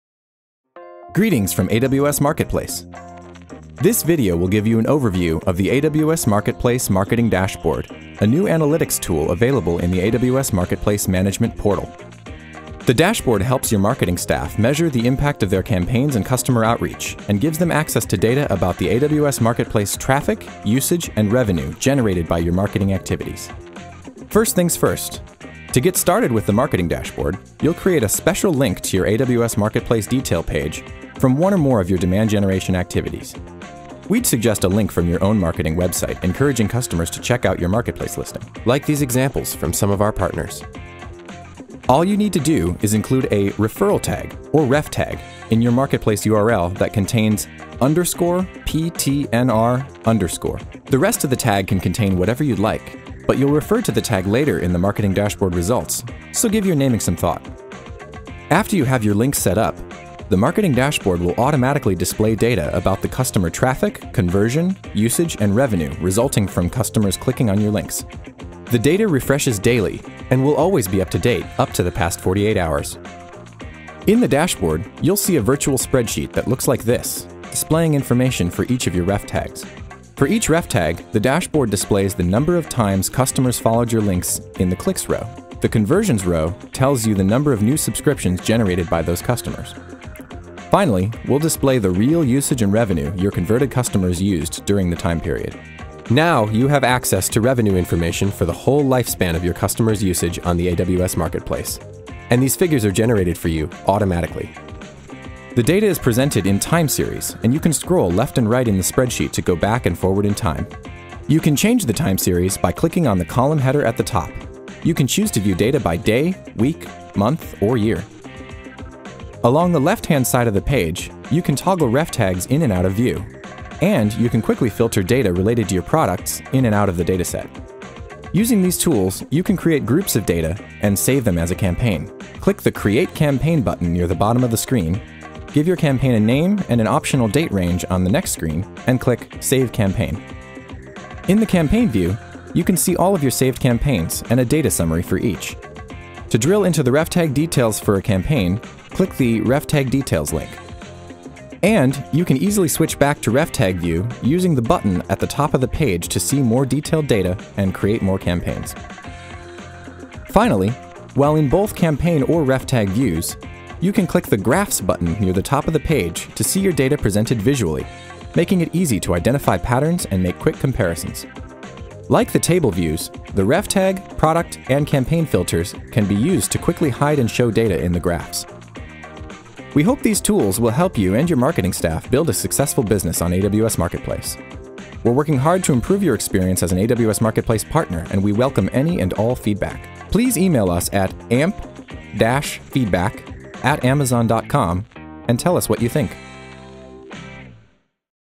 Getting Started with AWS Marketplace Marketing Analytics
This video introduces you to the Marketing Analytics Dashboard in the AWS Marketplace Management Portal. The analytics dashboard gives AWS partners access to powerful traffic, usage, and revenue data to help them run their marketing campaigns, and understand their marketing ROI.